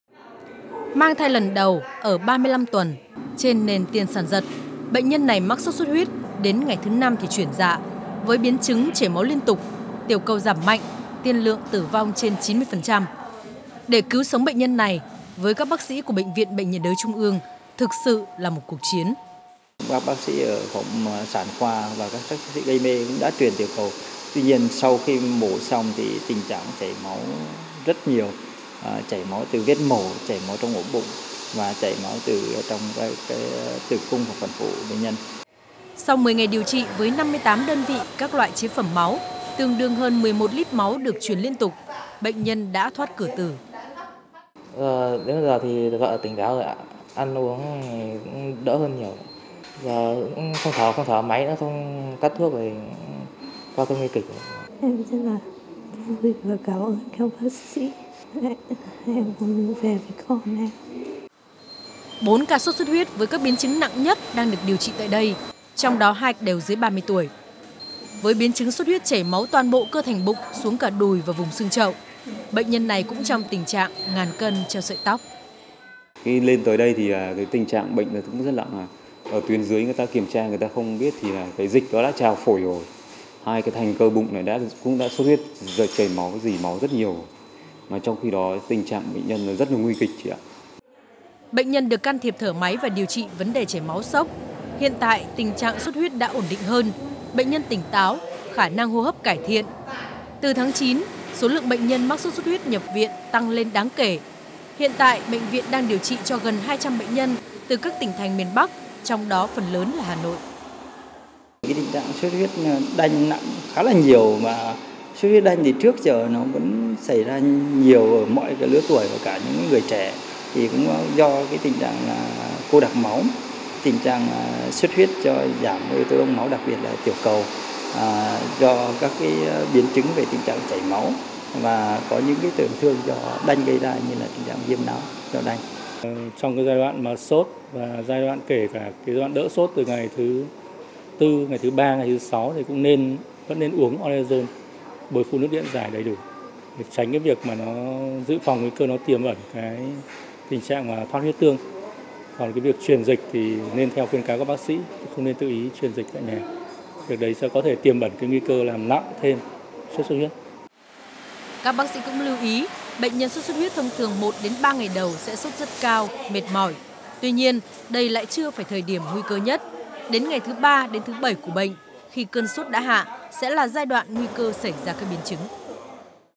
Chạy đua cứu sống bệnh nhân sốt xuất huyết nguy kịch| Tin Tức Dân Sinh
Tin Tức Dân Sinh | Không còn theo chu kỳ, dịch sốt xuất huyết năm nay đã tăng nhanh, đặc biệt tại Hà Nội số ca mắc tăng gấp 3 lần so với cùng kỳ năm ngoái. Đặc thù các bệnh nhân sốt xuất huyết nặng năm nay có diễn biến phức tạp với nhiều bệnh cảnh khác nhau.